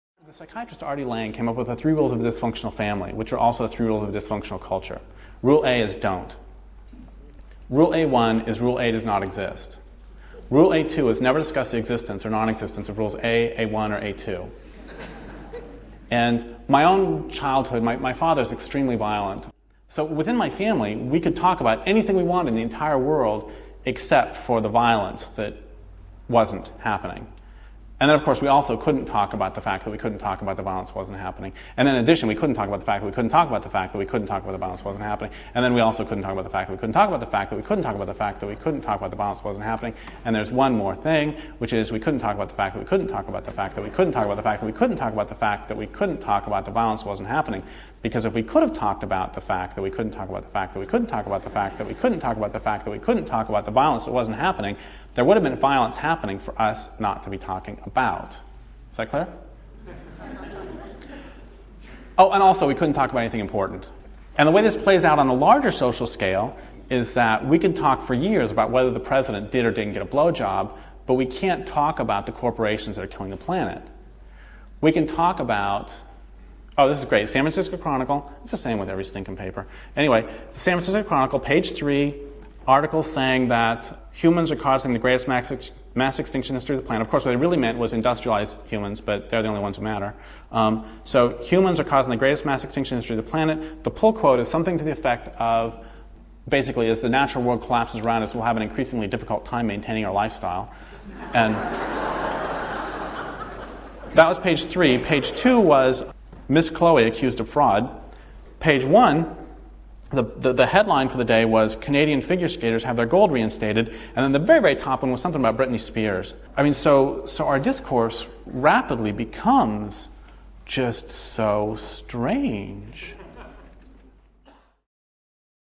violence
the three rules of a dysfunctional culture

audio ganked from derrick jensen's 3-cd set
"the other side of darkness"